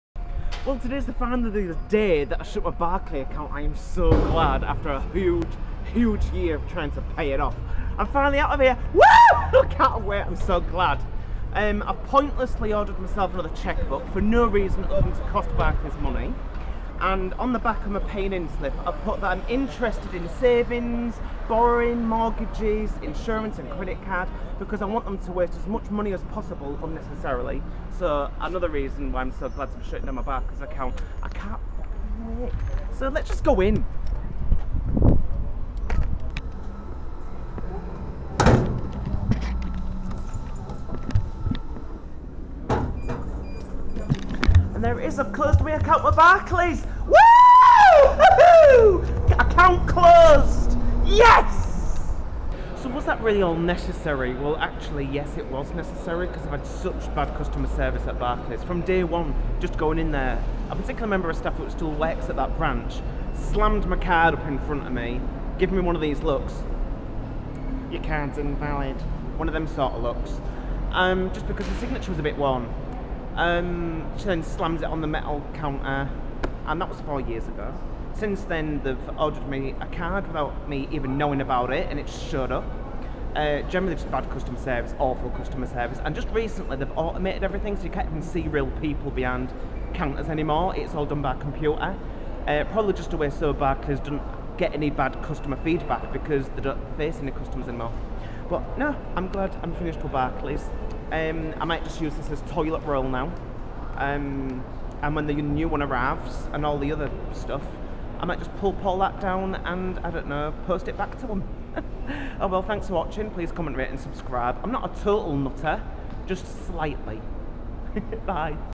Bye Bye Barclays
Awful staff, awful service, awful bank. I am so happy I won't have to go in there ever again. Even head office is rubbish and lied, sending me letters saying they tried to call and update me on the issues raised but my phone number was wrong... I have had the same number since 2001 but when they called in the overdraft... they found my number! I have ticked everthing so when it arrives in the post I will RETURN TO SENDER! As I live opposite a post box and show them that I can waste their time and money as they have mine.